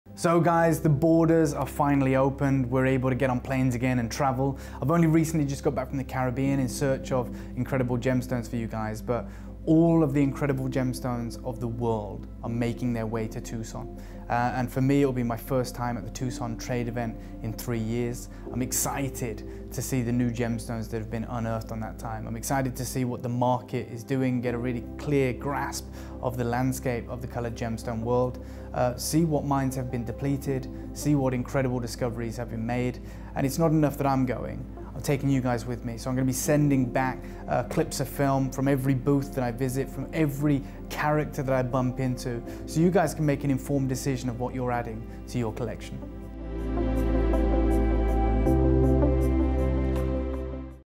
Join Dave Troth at the Tucson Trade Fair
David Troth tells us about his upcoming visit to the Tucson Trade Fair